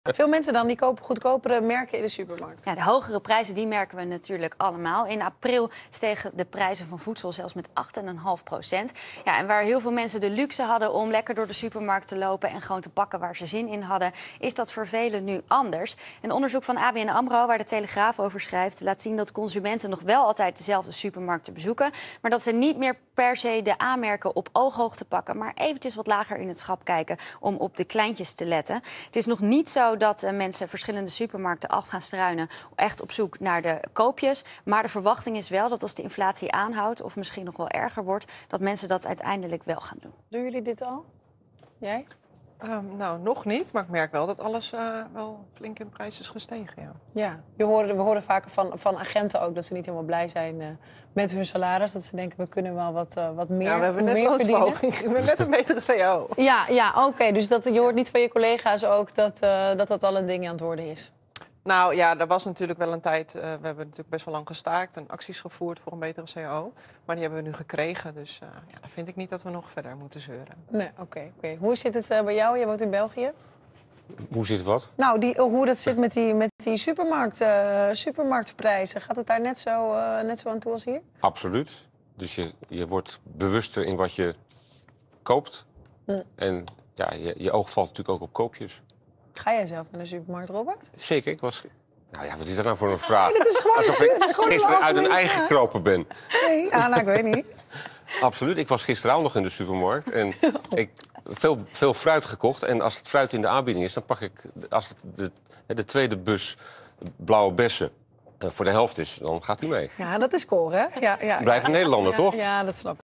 Consumenten grijpen vaker naar huismerk, maar blijven dezelfde supermarkt bezoeken
De inflatie heeft ervoor gezorgd dat mensen in de supermarkt kiezen voor goedkopere producten en minder snel naar A-merken grijpen.